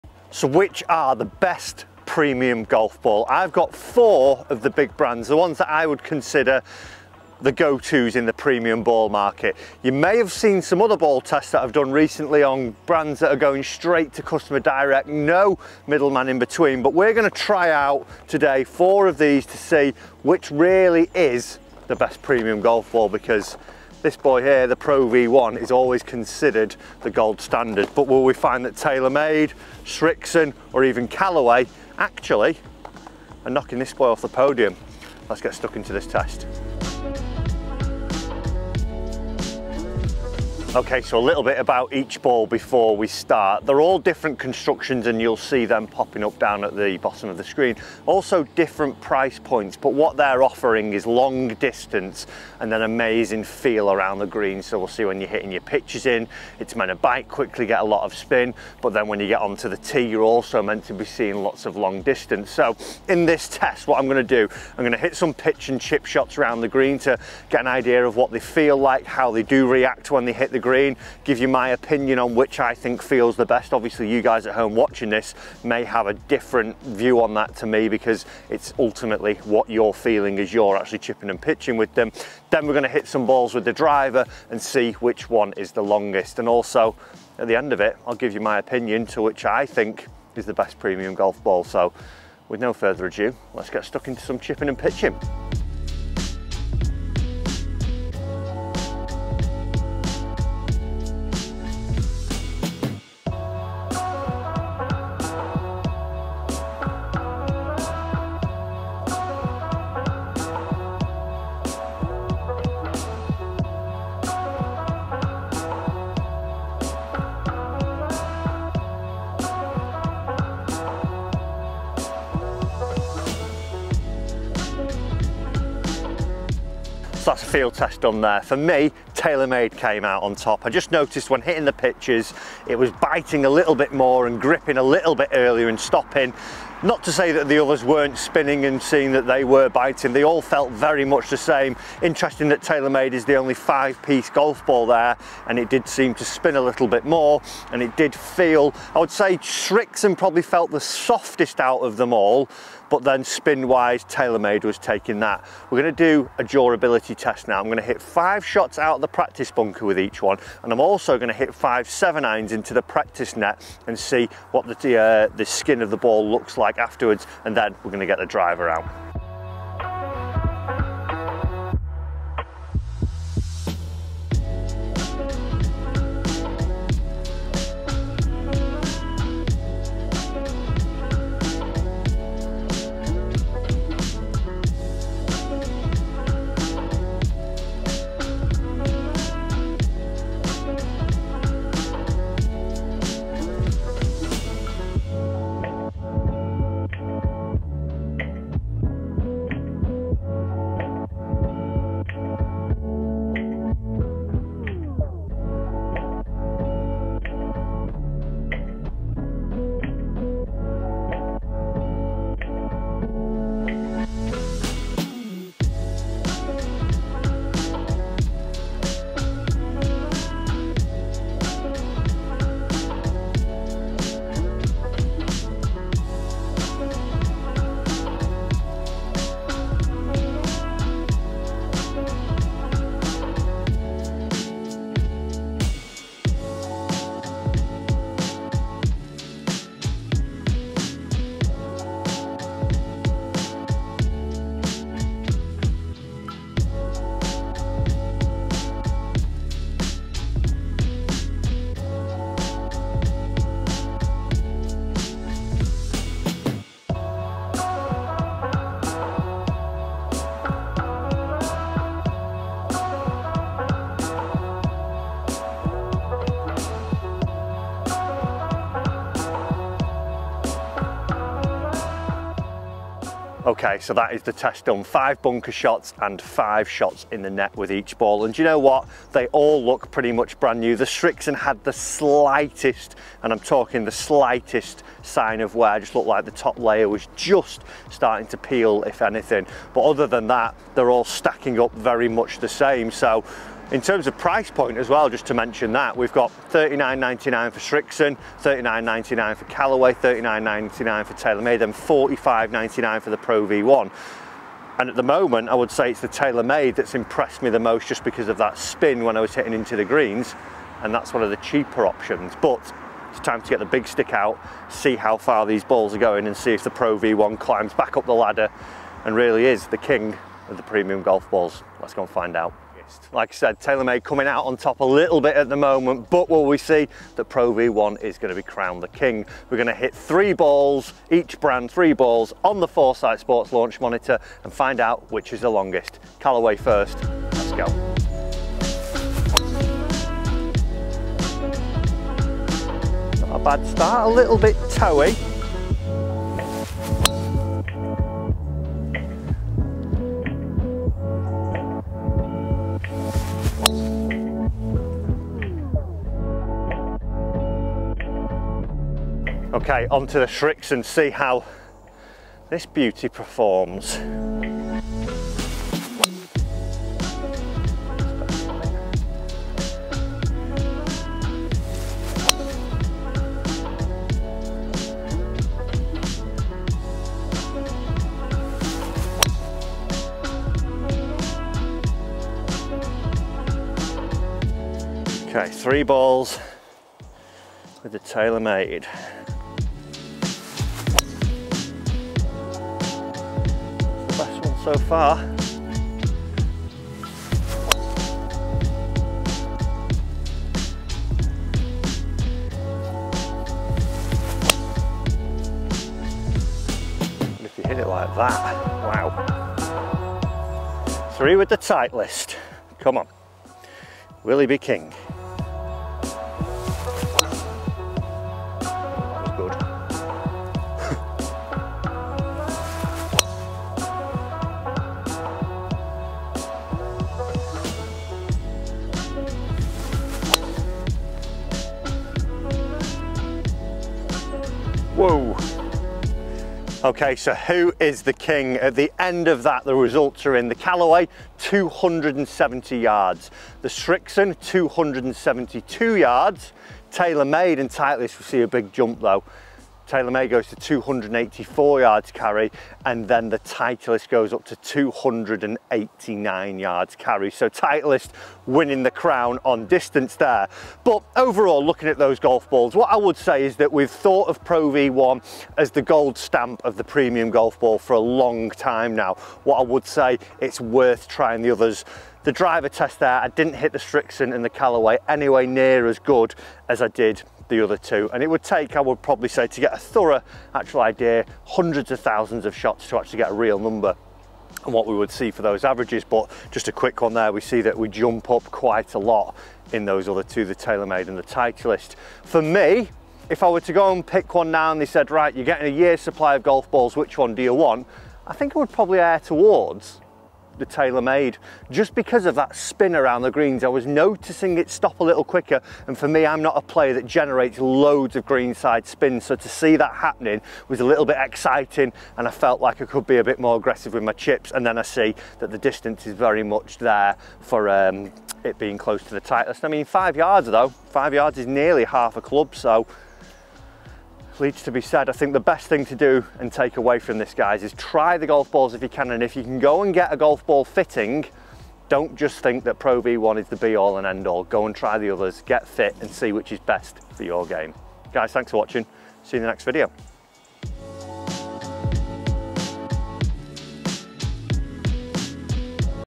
Is PRO V1 REALLY The BEST Premium Golf Ball?
We have known the Titleist Pro V1 as the gold standard of premium golf balls, but is it really the best? In this golf video Matt Fryer Golf puts it to the test against the other big golf brands competing in this space. Find out how Matt Fryer Golf gets on and if he thinks Pro V1 is the best premium golf ball.